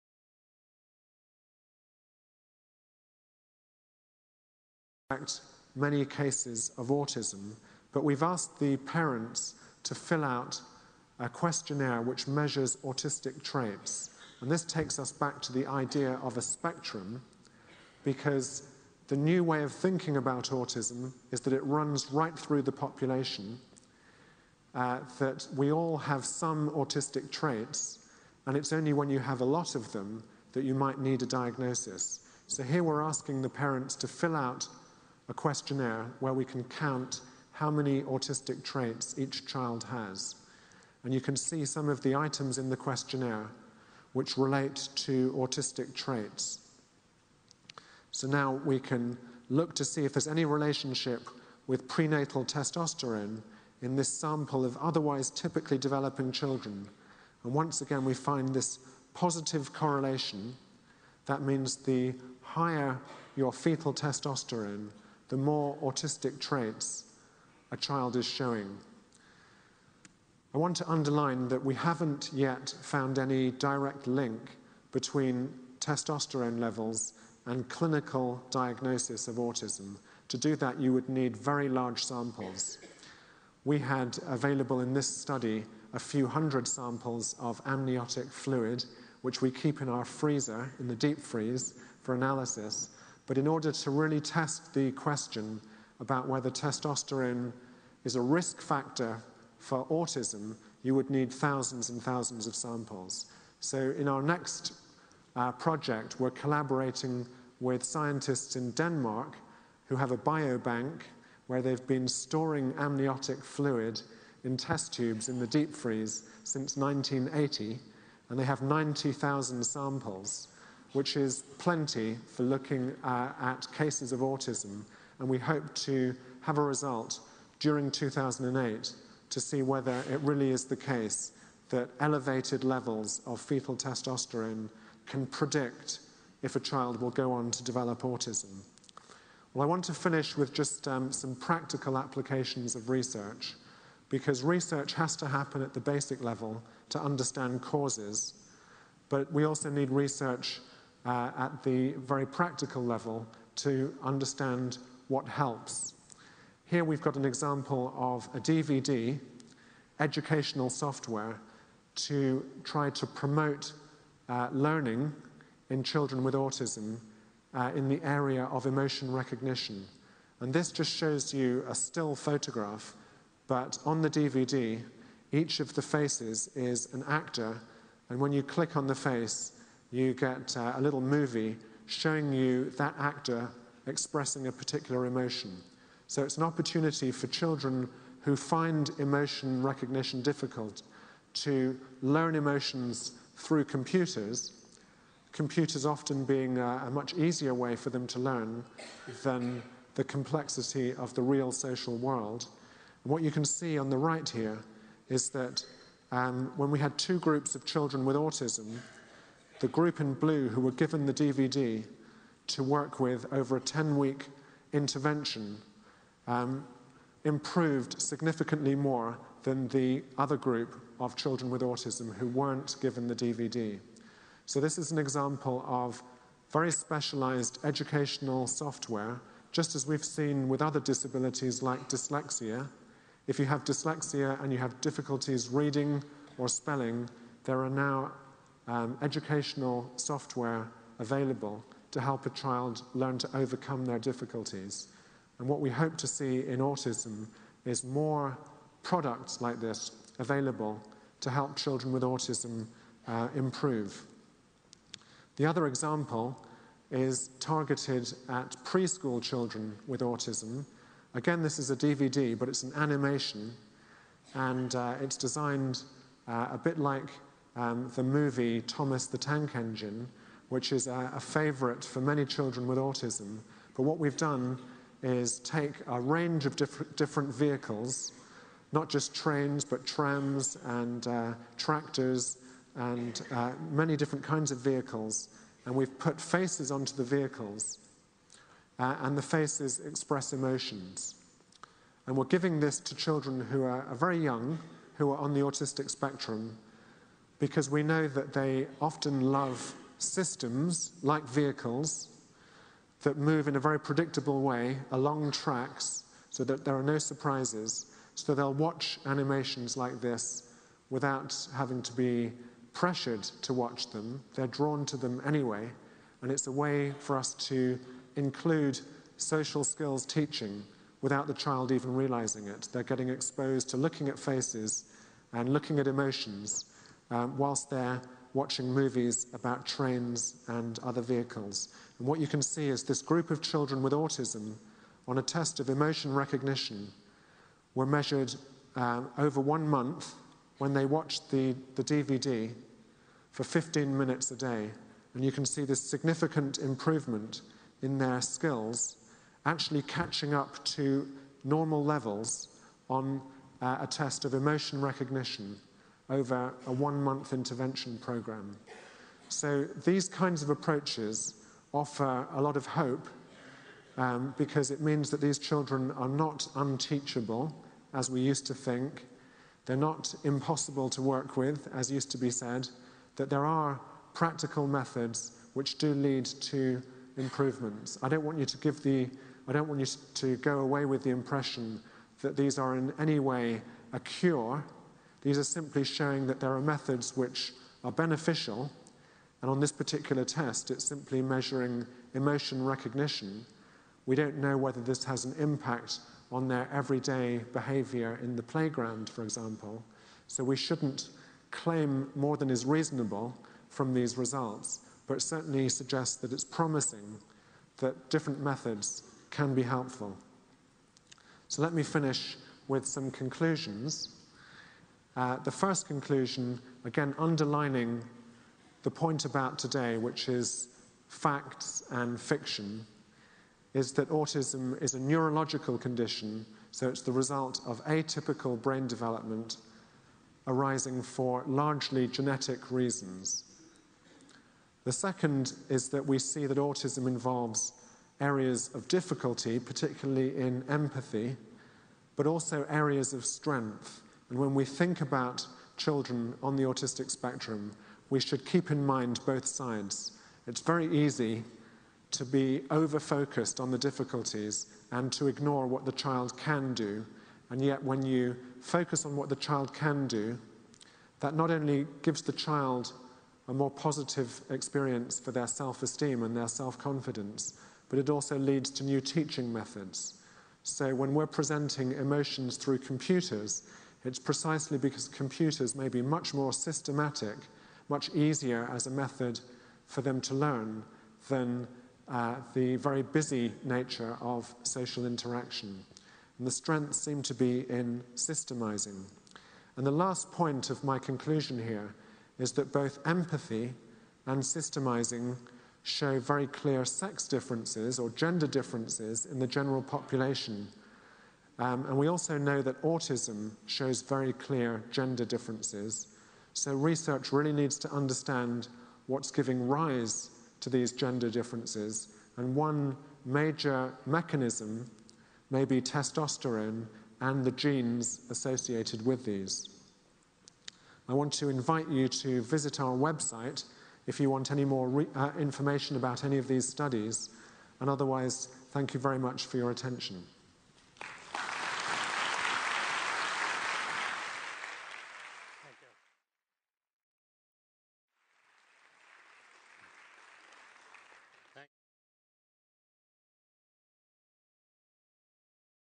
Autism Research: Fact and Fiction Part 6
Simon Baron-Cohen is Professor of Developmental Psychopathology at the University of Cambridge and Fellow at Trinity College, Cambridge. He is Director of the Autism Research Centre (ARC) in Cambridge and holds degrees in Human Sciences from New College, Oxford, a Ph.D. in Psychology from UCL and a M.Phil in Clinical Psychology at the Institute of Psychiatry. Dr. Baron-Cohen is also Director of CLASS (Cambridge Lifespan Asperger Syndrome Service), a clinic for adults with suspected AS.